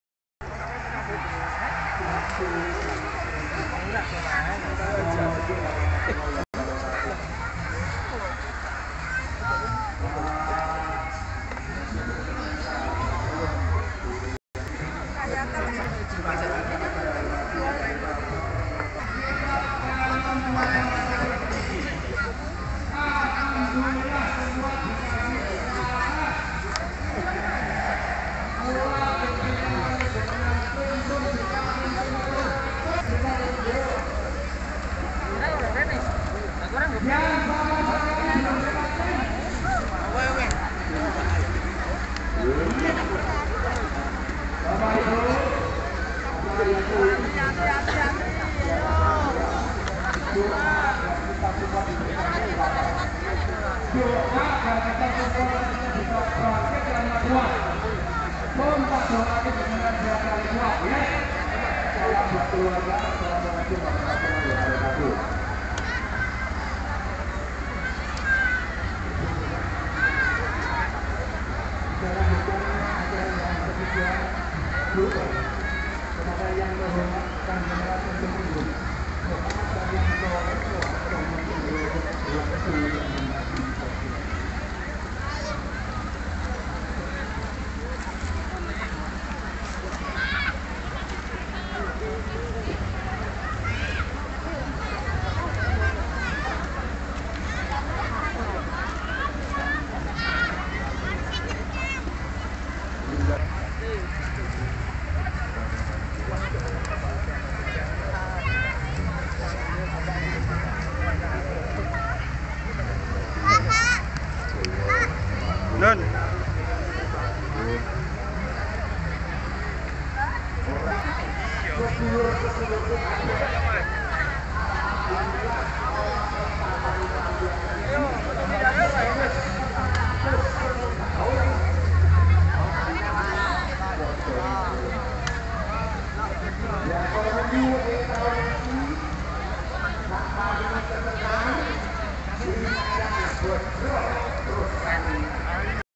Lautan manusia acara puncak saparan sebaran apem
Acara puncak Saparan sebaran apem yaqowiyu di Jatinom Klaten